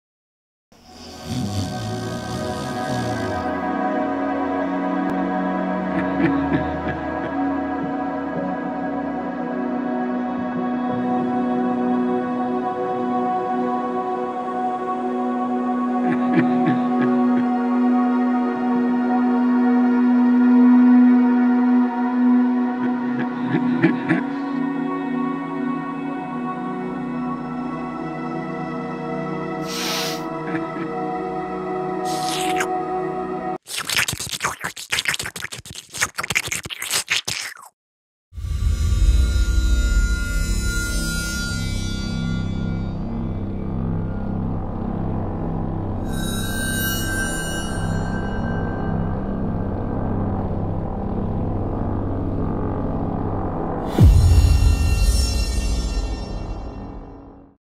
Inside Sleepy Joe Biden's dream.
Sleepy Joe has a dream.  A splash with Chocolate Swirls.  A dip with Cake and Waffle Cones.  A sniff of sweet smelling vanilla.  A lick of tasty Mint Chocolate Chip.  Joe Loves Ice Cream!

WAKE UP!